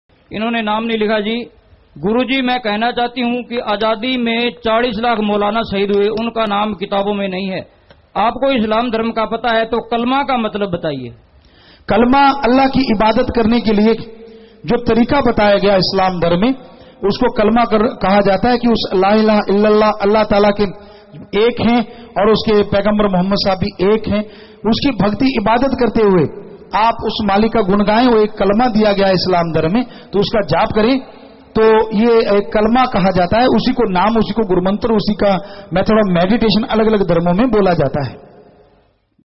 Q- 536 कलमा का मतलब बताइए ? आजादी में 40 लाख मौलाना ... Answered By Saint Dr. MSG
Q. गुरु जी मैं कहना चाहती हूं कि आजादी में 40 लाख मौलाना शहीद हुए उनका नाम किताबों में नहीं है आपको इस्लाम धर्म का पता है तो कलमां का मतलब बताइए ?
Saint Dr. Gurmeet Ram Rahim Singh Ji Insan